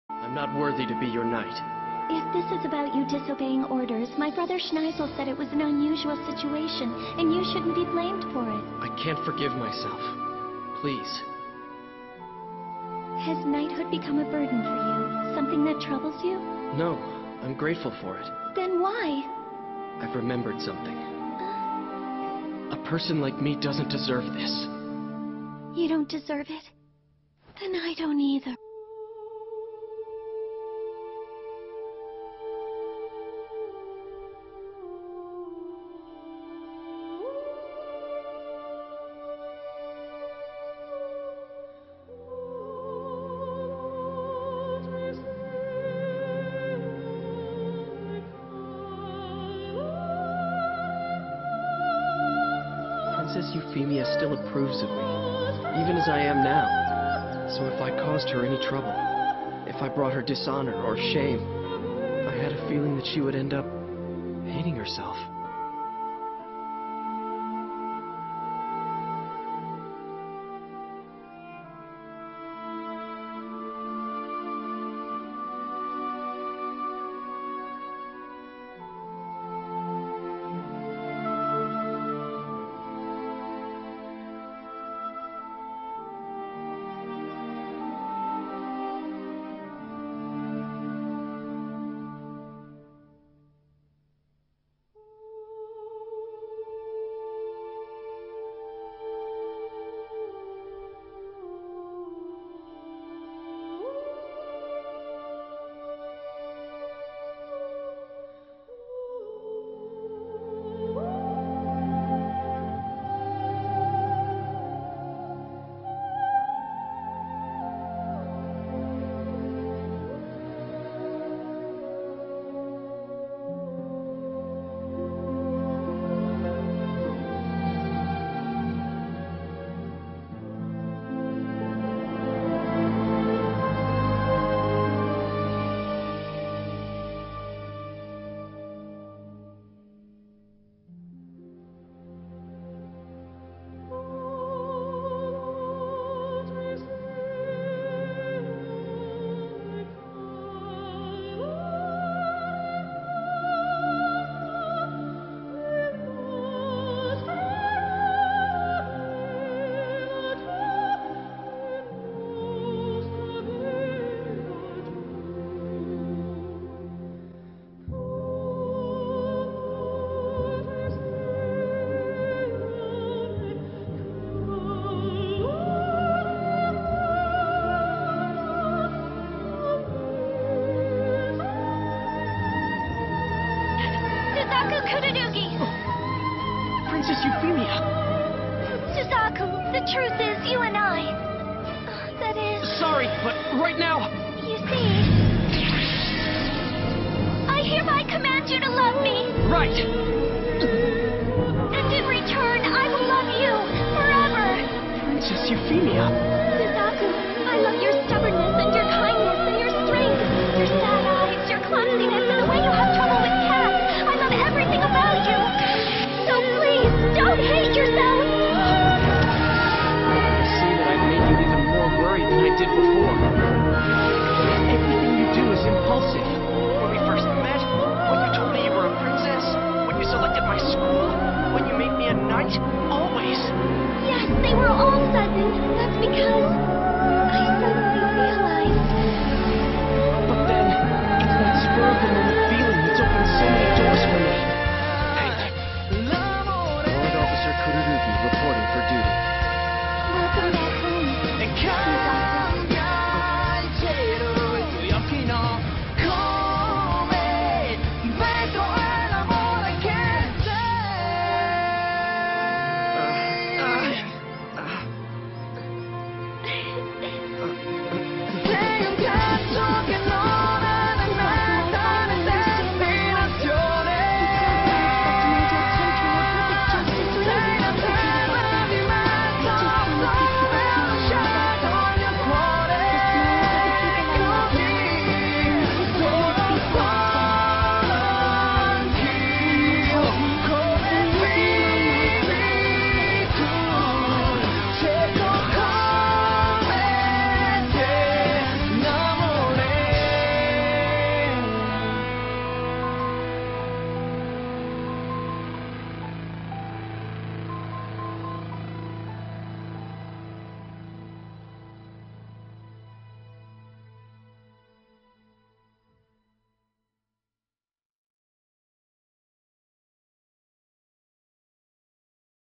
Suzaku Euphemia
I made this for a contest, but I don't think I'll enter it...turned out real bad.

Plot-
This Takes place when Suzaku quits being Euphemia knight. She thinks about thier time together and realizes she can't live with out him by her side.